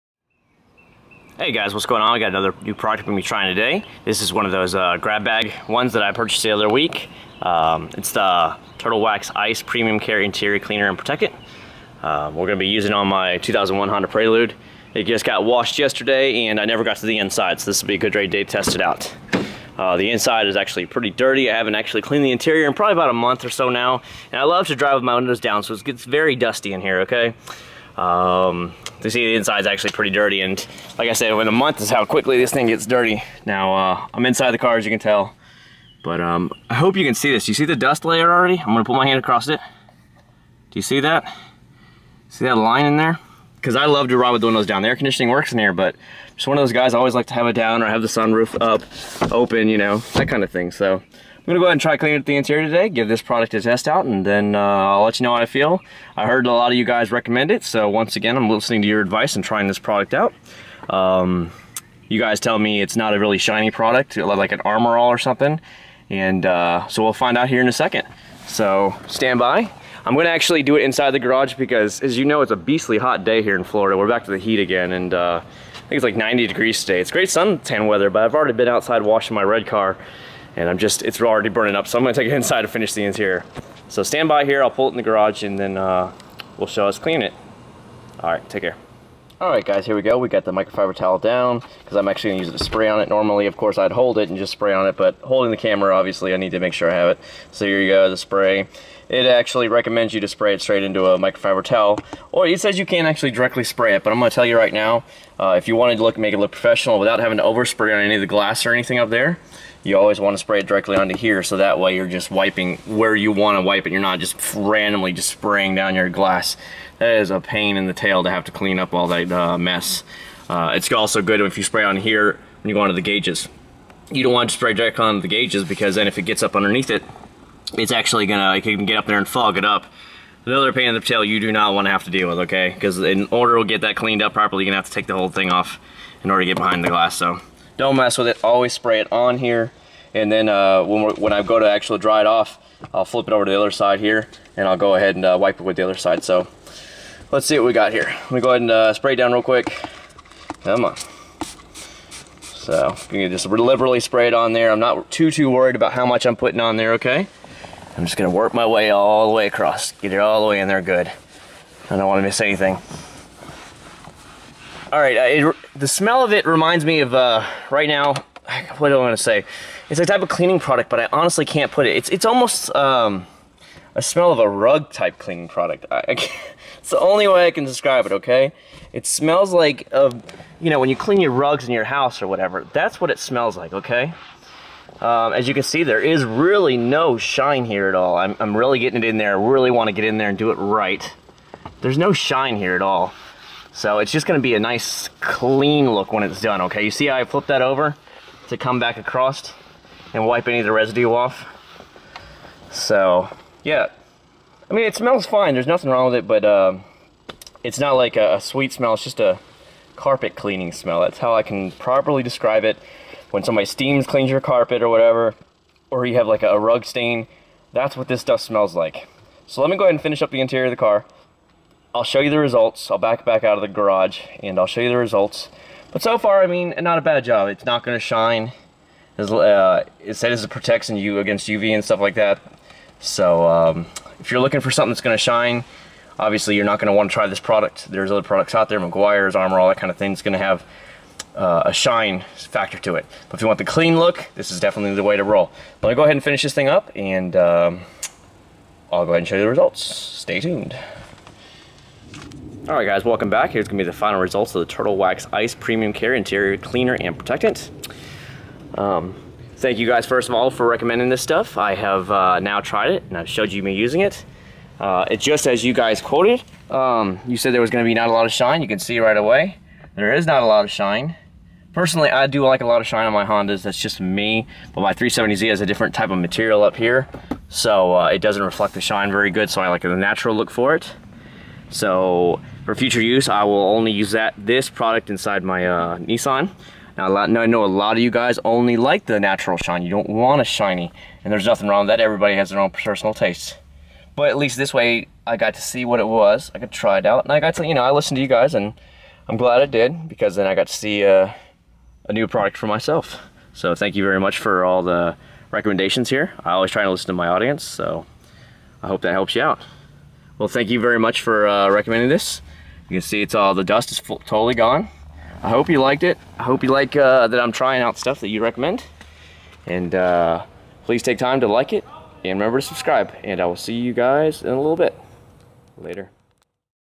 Turtle Wax Ice Premium Care Interior Cleaner and Protectant Review and Test Results on my Prelude
Thank you very much for letting me know each product you wanted to see. I will always listen to you, and if I don't, feel free to unsub! haha Eitherway, I hope you enjoyed and I hope you learned a lot!